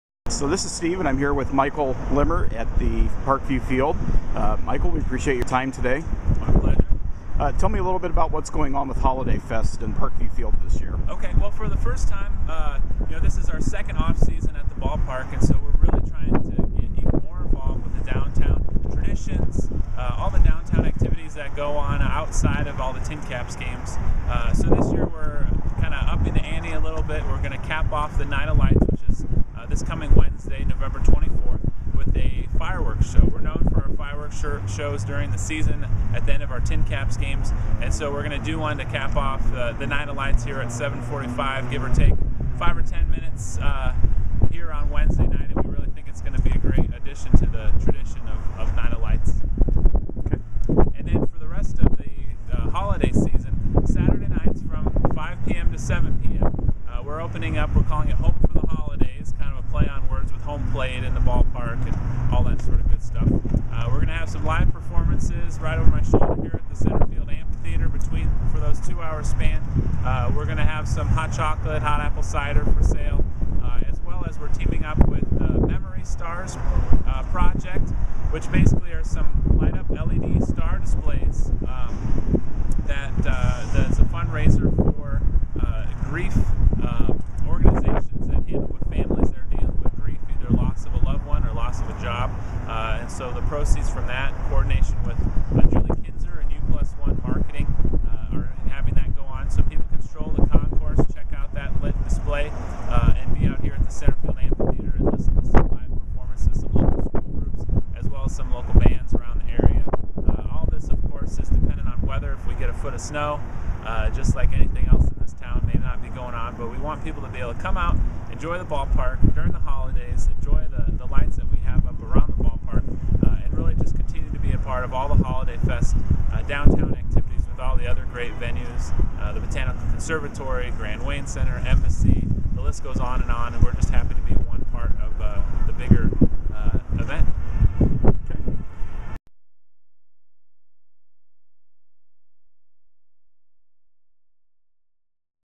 2010/11/22: TinCaps HolidayFest preview
We talk with the TinCaps Michael Limmer, Vice President of Marketing about the downtown HolidayFest activities at Parkview Field in Fort Wayne Indiana.